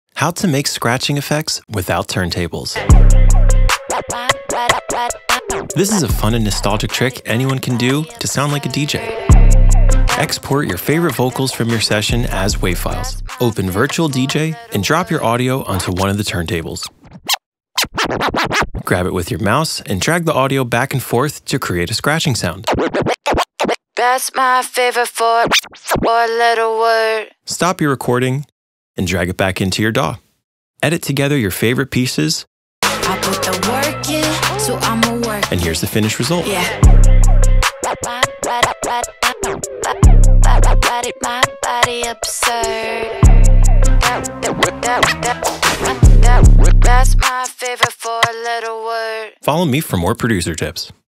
DJ Scratching Tutorial🔥 Without Turntables 🚫
How To Create DJ Scratching 🔥 Without Turntables 🚫
Virtual DJ software is the easiest way to scratch audio with your computer for free!